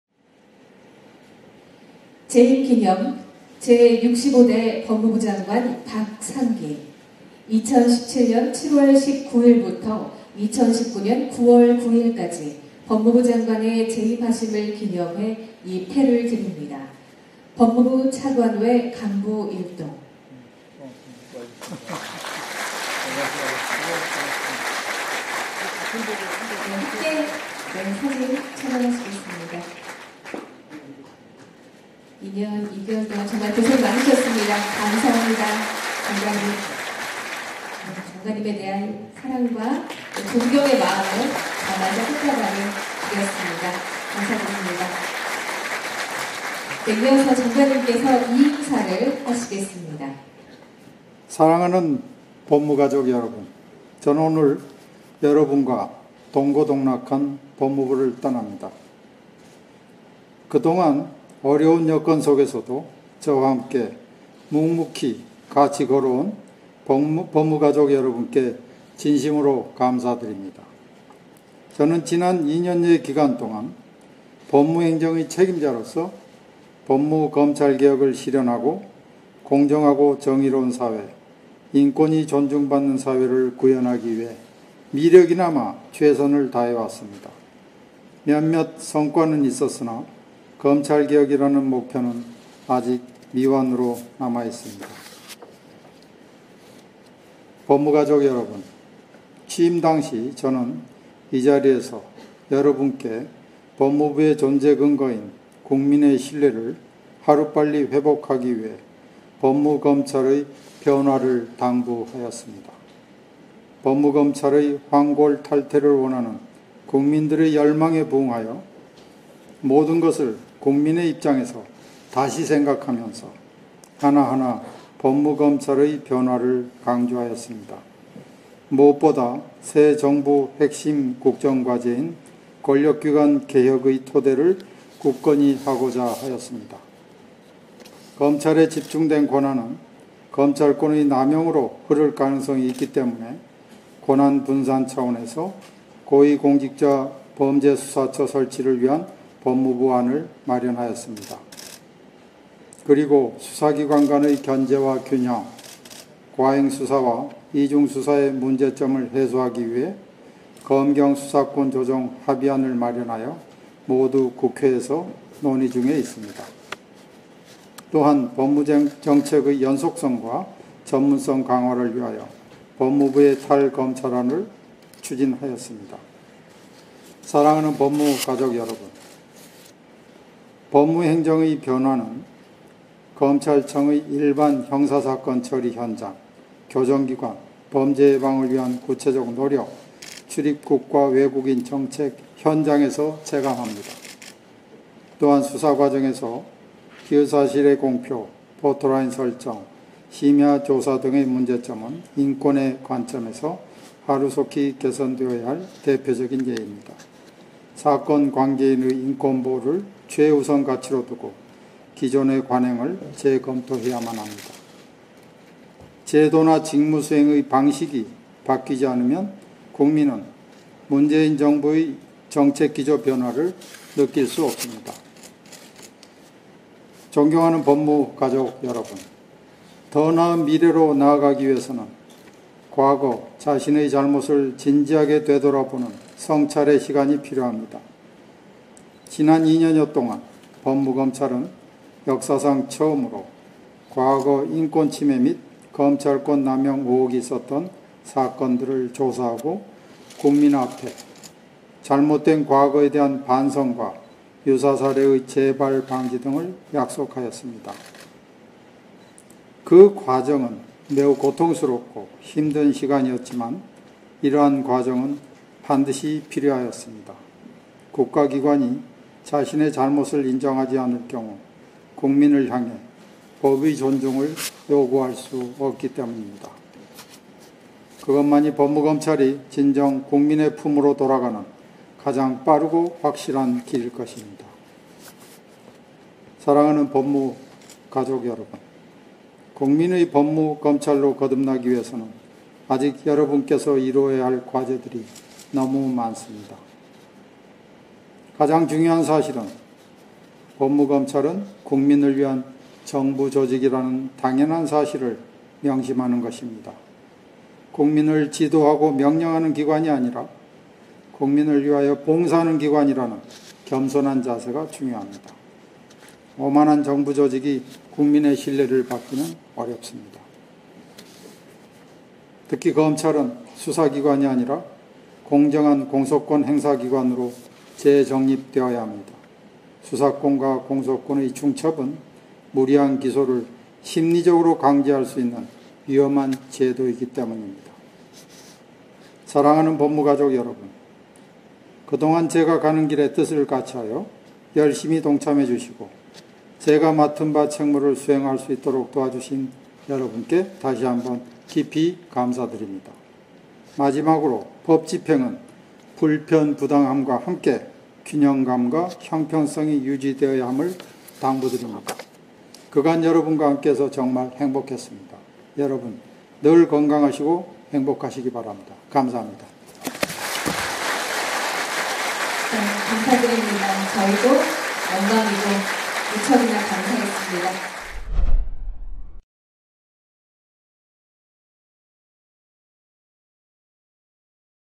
박상기 퇴임, "검찰개혁 미완…오만한 조직 신뢰받기 어려워" / 연합뉴스 (Yonhapnews)
(서울=연합뉴스) 박상기 법무부 장관의 퇴임식이 오늘 오후 3시 정부과천청사 대강당에서 열렸습니다. 박 장관은 이임사에서 검찰은 수사 기관이 아니라 공정한 공소권 행사기관으로 남아야 한다"며 "검찰개혁이라는 목표는 아직 미완으로 남았다"고 밝혔는데요. 자세한 내용 영상으로 확인하세요.